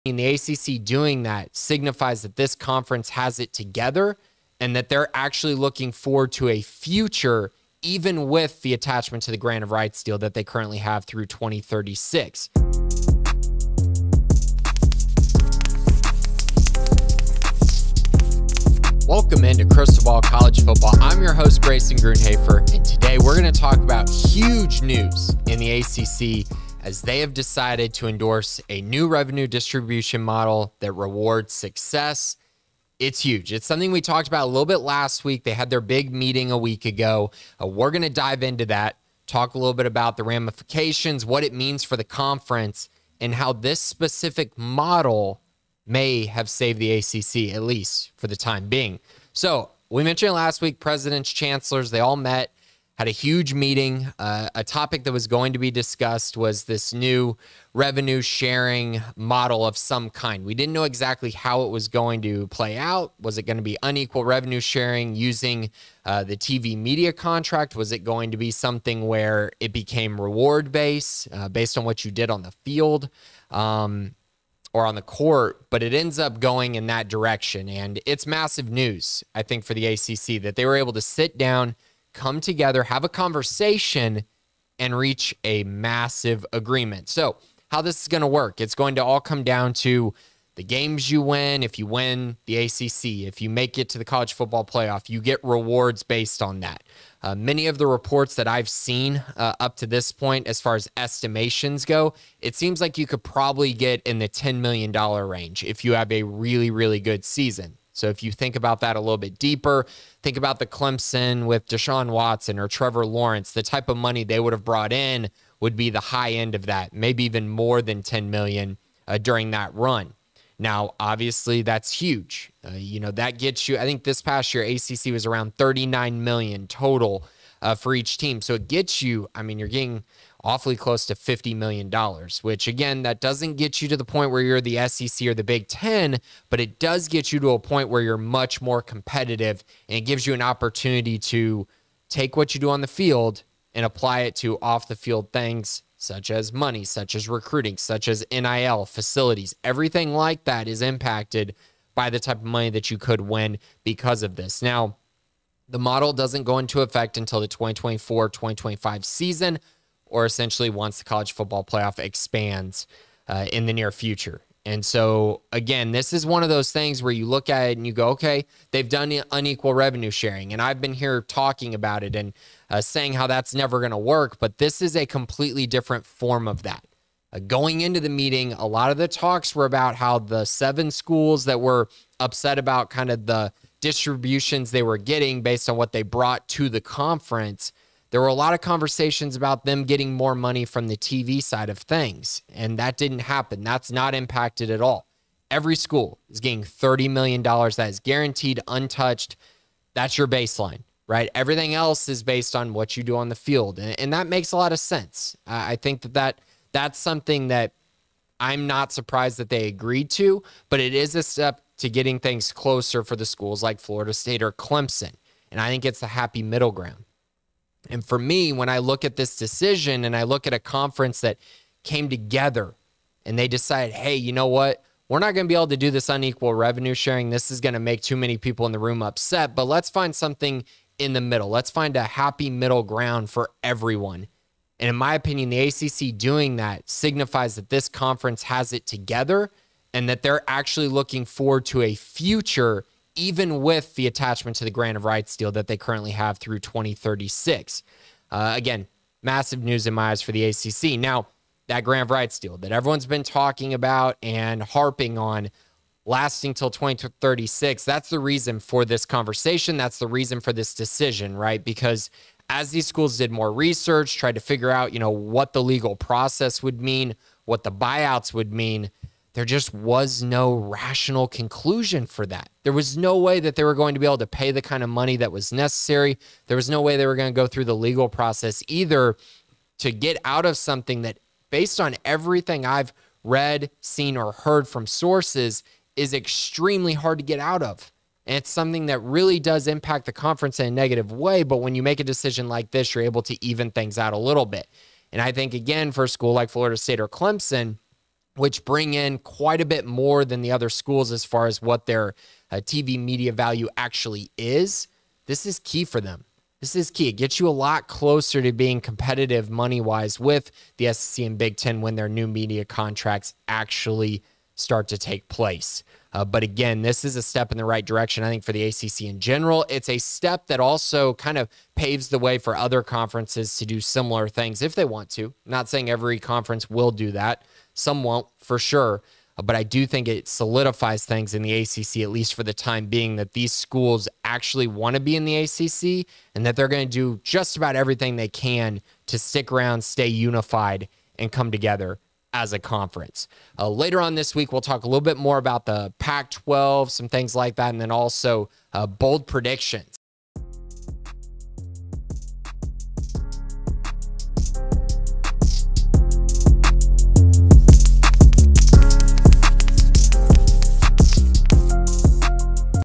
ACC Saved Itself with Merit-Based Revenue Sharing | Conference Realignment Media Deals
ACC Saved Itself with Merit-Based Revenue Sharing | Conference Realignment Media Deals with Grayson Grundhoefer on Crystal Ball CFB

365 Sports, SicEm365, College Basketball 365, NFL on 365, and Dallas Cowboys 365.